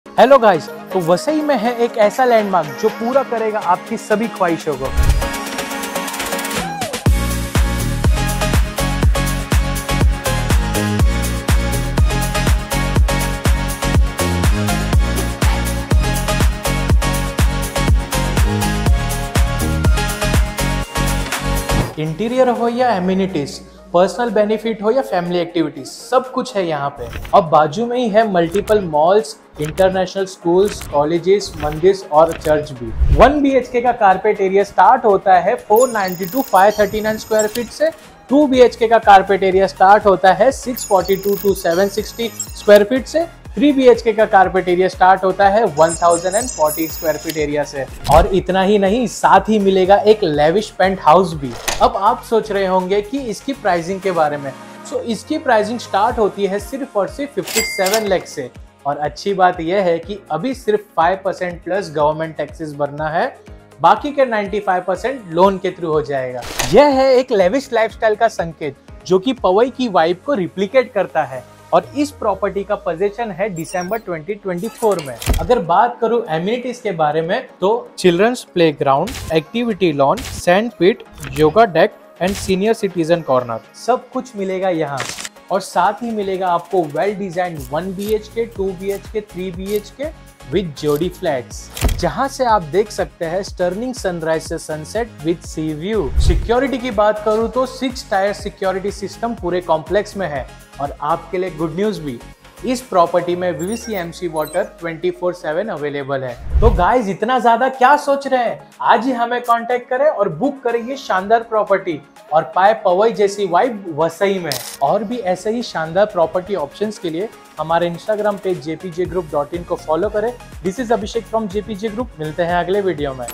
PREMIUM LIFESTYLE ALERT 🚨1,2,3 BHK, Jodi Homes & Pent Houses starting at 57 lacs onwards.
PREMIUM LIFESTYLE ALERT 🚨
1,2,3 BHK, Jodi Homes & Pent Houses starting at 57 lacs onwards.

Possession Date: 💫
December 2024

Project Specifications 🤩

●Beautiful & Grand Elevation which replicates Powai Vibe.
●Wodden finished flooring in all bedrooms.
●Spacious & Premium finished Air Conditioned entrance lobby and typically floor lift lobbies.
●Premium Fittings from Cera and jaguar in Powder room And washroom
●Pre-installed video door phones.
●Internet provision in each apartment.

Stunning Amenities 😍

● Children’s  Playground
● Toddler's play area
● Activity Lawn
● Sand Pit
● Yoga Deck
● Senior Citizens Corner

We have P.G(Playground) Reservation area beside project.

Investment USP 🤑

● Most Premium and Lavish Project in the Heart of Vasai.
● Larger Apartment layouts
● 6 Tire Security system in whole complex
● VVCMC Water Supply 24*7
● STP Water 24*7(only in flush)
● Balcony Homes
● Pay only 5%+ Govt. Taxes now & Rest 95% Loan.

Follow @jpjgroup.in if you ❤️ real estate
YouTube 👉🏼 jpj group